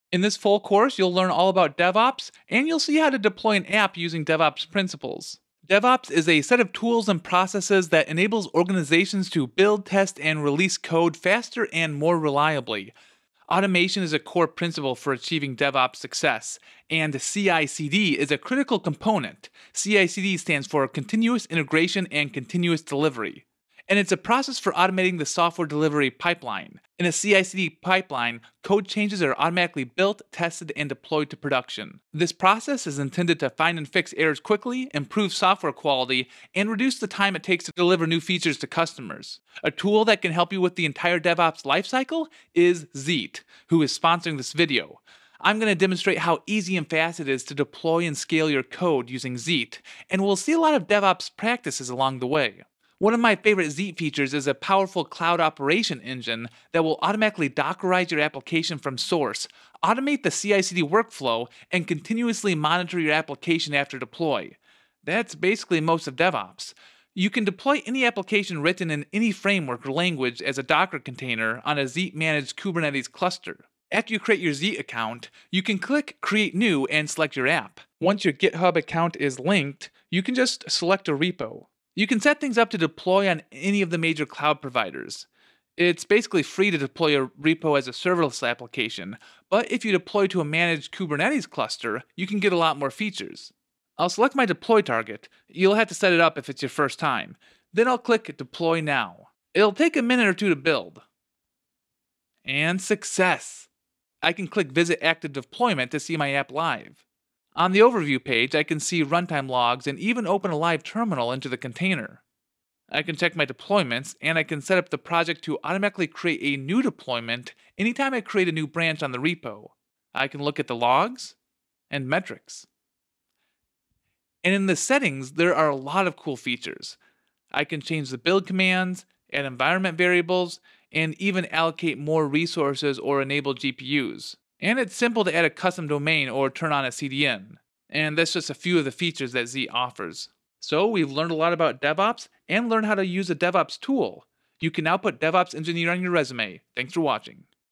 Learn DevOps & CI/CD – Full Course in 2.5 Minutes
With this full DevOps course, you'll become a DevOps engineer in no time. 😉 Learn the basics of DevOps plus learn how to deploy an app using Zeet, an all-in-one DevOps platform.